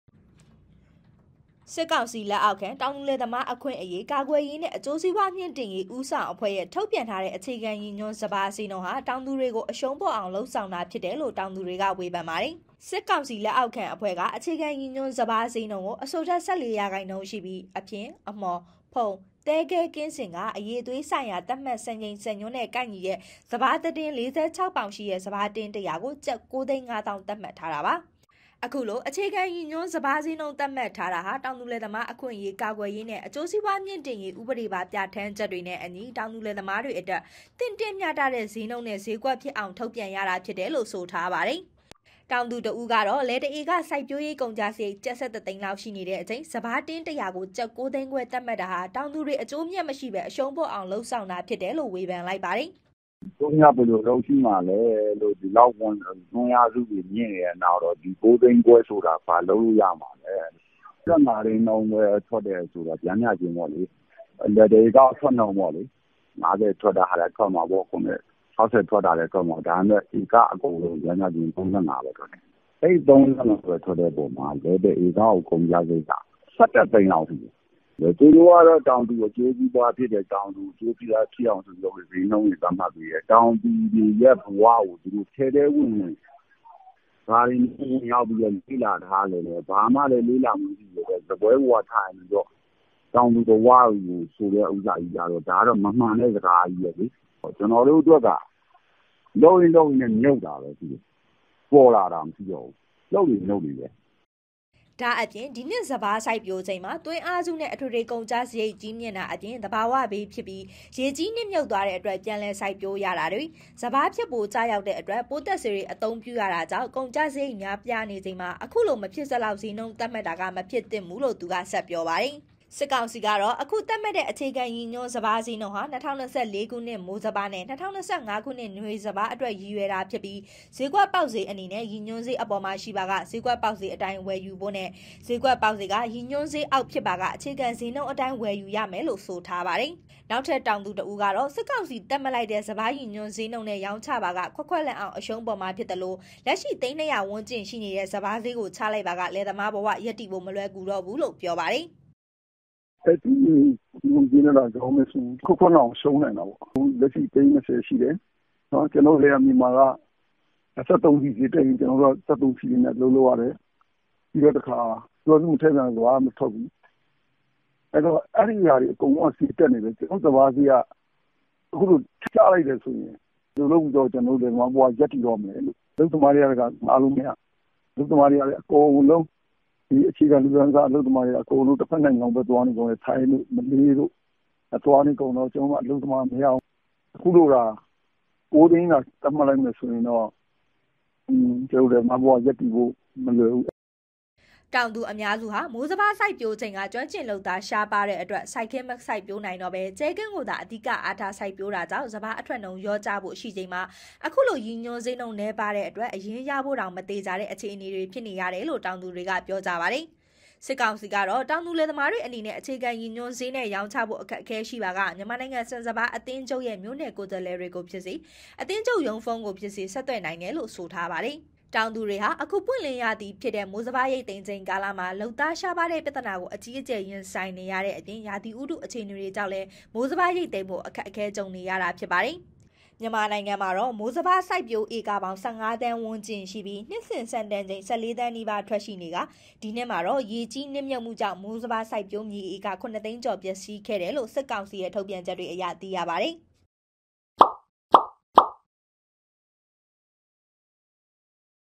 TLT Tv||- စစ်ကောင်စီရဲ့ အခြေခံရည်ညွှန်း စပါးဈေးနှုန်း သတ်မှတ်မှု တောင်သူတွေ ဝေဖန်
စစ်ကောင်စီ လက်အောက်ခံ တောင်သူလယ်သမား အခွင့်အရေး ကာကွယ်ရေးနဲ့ အကျိုးစီးပွား မြှင့်တင်ရေး ဦးဆောင် အဖွဲ့ရဲ့ ထုတ်ပြန်ထားတဲ့ အခြေခံရည်ညွှန်း စပါးဈေးနှုန်းဟာ တောင်သူတွေကို အရှုံးပေါ်အောင် လုပ်ဆောင်တာ ဖြစ်တယ်လို့ တောင်သူတွေက ဝေဖန်ပါတယ်။